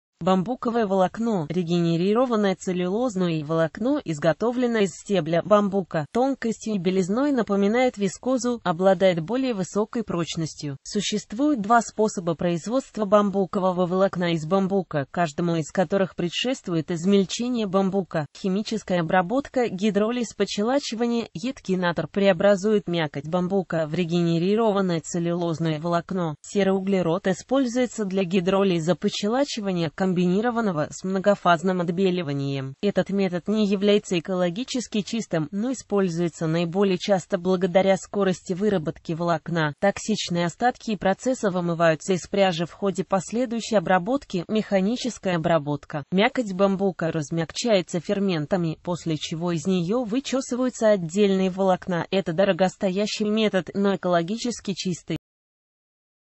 Бамбуковое волокно
Бамбу́ковое волокно́ — регенерированное целлюлозное волокно, изготовленное из стебля бамбука.Тонкостью и белизной напоминает вискозу, обладает более высокой прочностью.Существуют два способа производства бамбукового волокна из бамбука, каждому из которых предшествует измельчение бамбука.Химическая обработка – гидролиз-подщелачивание.